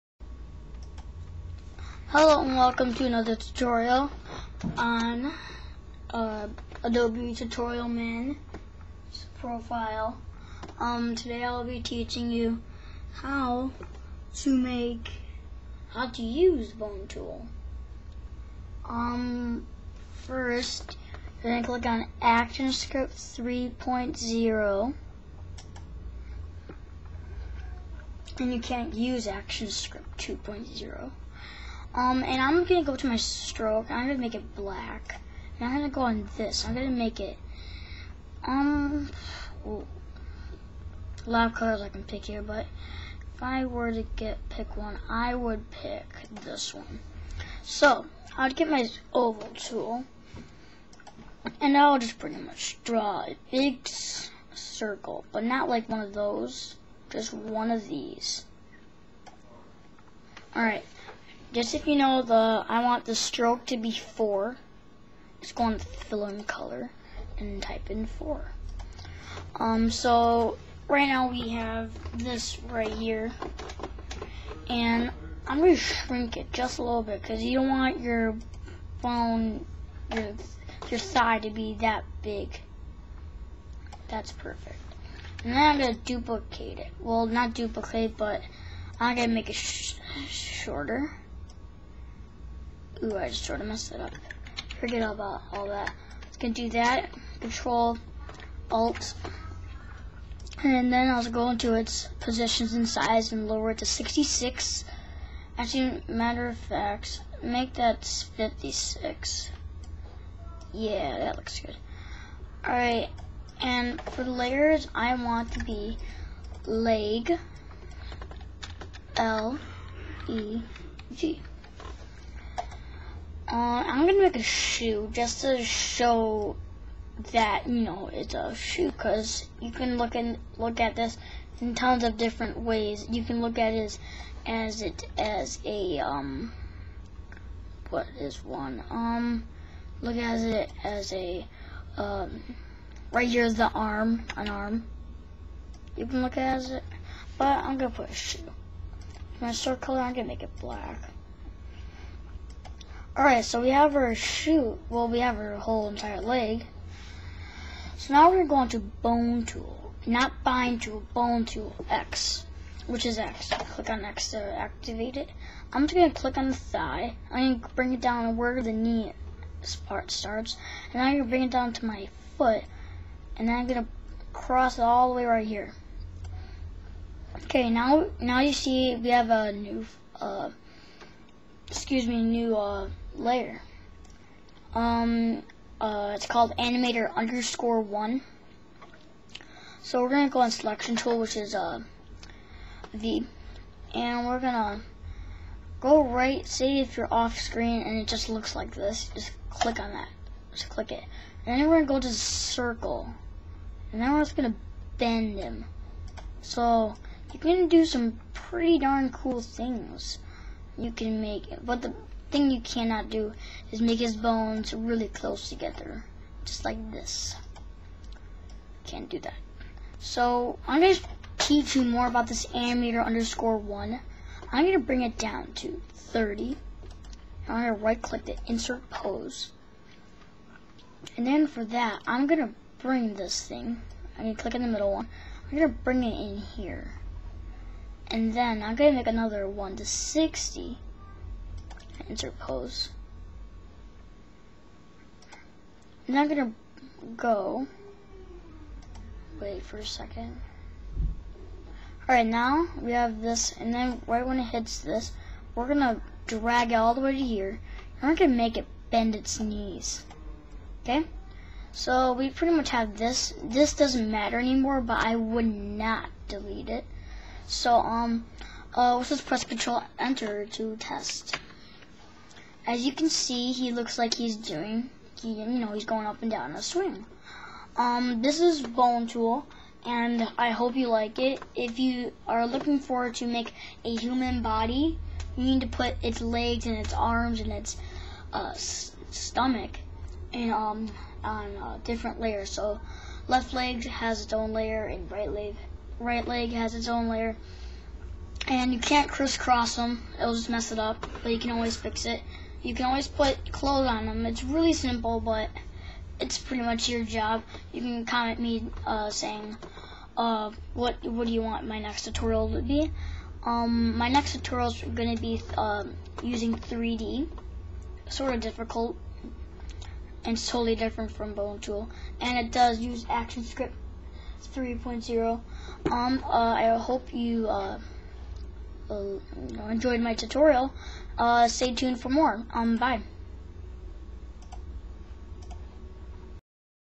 Flash CS4 Tutorial On How To Use Bone Tool
I will teach you how to use bone tool i will show you what to do and what not to do. no actionscripting and we are using Actionscript 3.0!!!